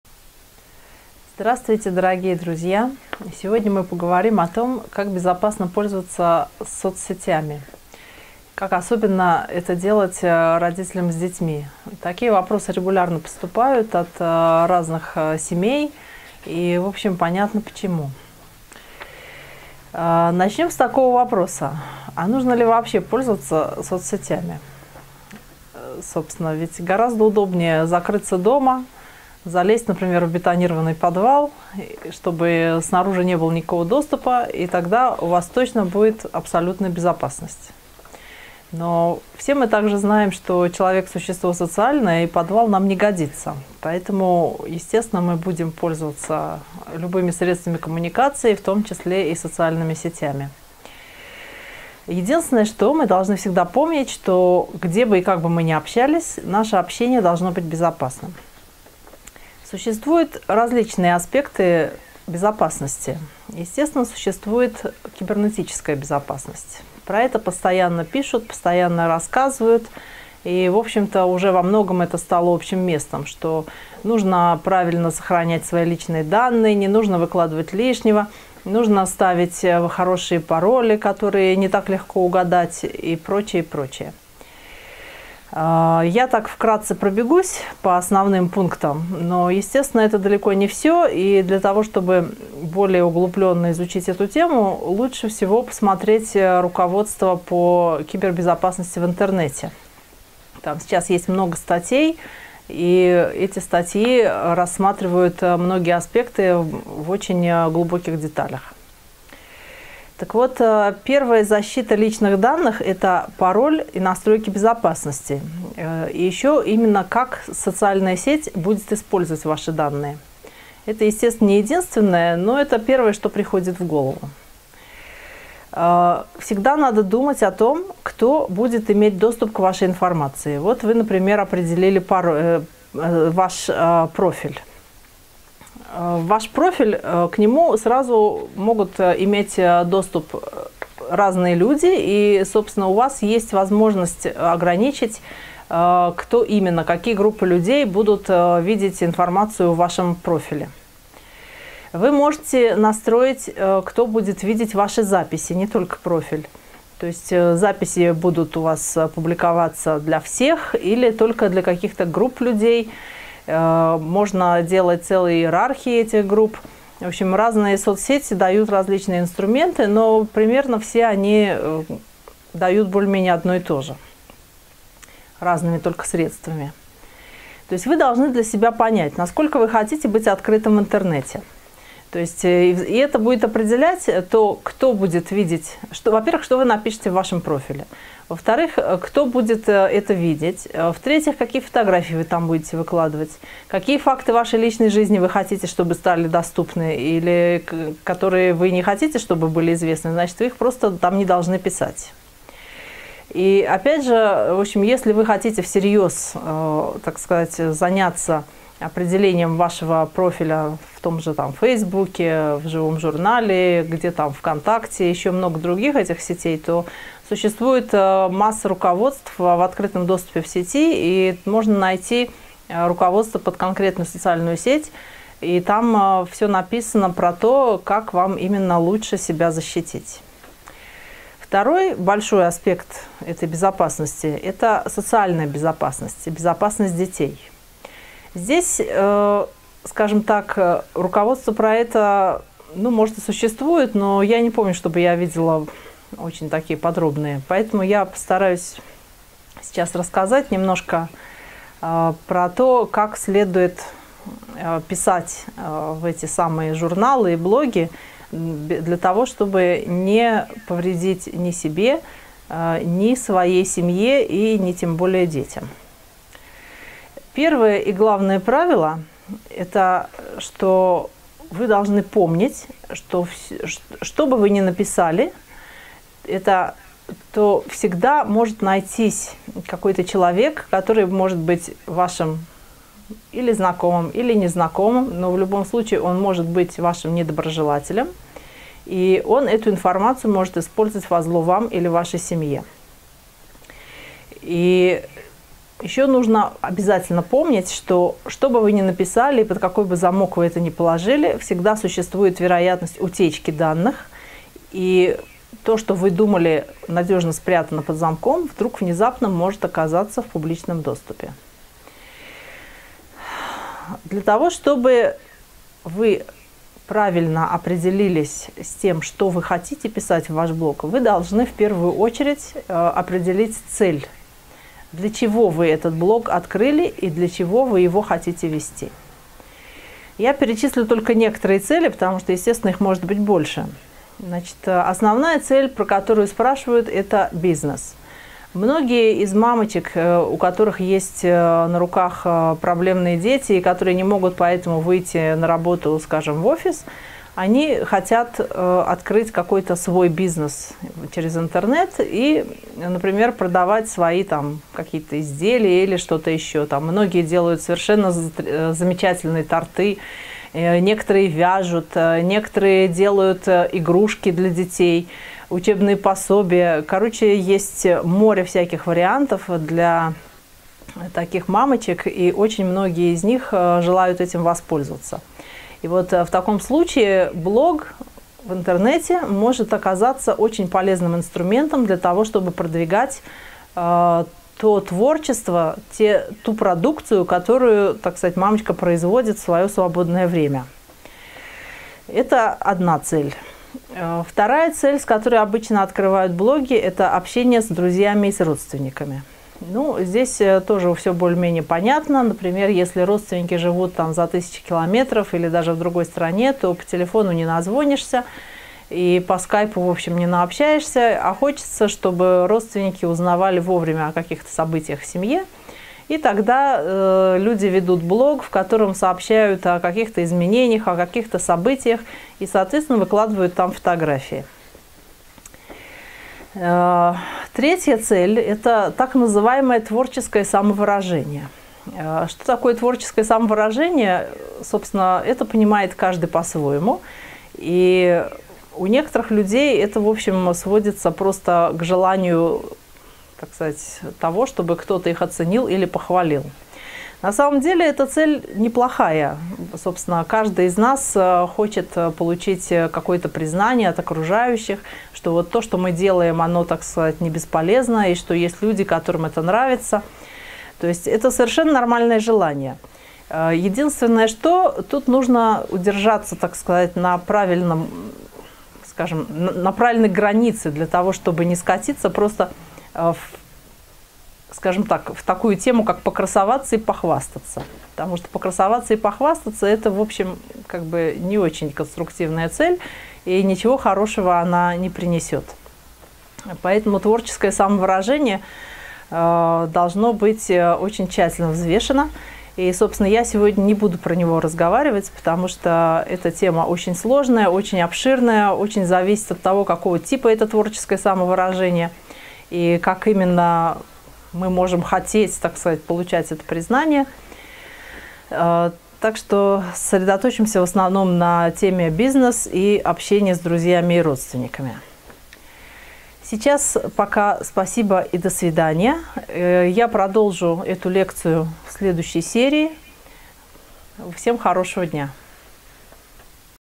Безопасность при взаимодействии с соц.сетями. Часть 1. Йосефа Эйхель. Центр BEER DAVID, Иерусалим
Нужно ли пользоваться соц.сетями в современном мире?
Аспекты безопасности. Рассказывает Йосефа Эйхель, ведущий специалист Центра BEER DAVID

1:50 - кибербезопасность
3:50 - социальная безопасность  и безопасность детей
4:30 - помнить о возможности взлома
5:13 - необходимость определить цель своей странички
5:34 - бизнес, как цель личной страницы
6:34 - общение с друзьями и родными, как цель личной страницы
7:06 - творческое самовыражение через соц.сети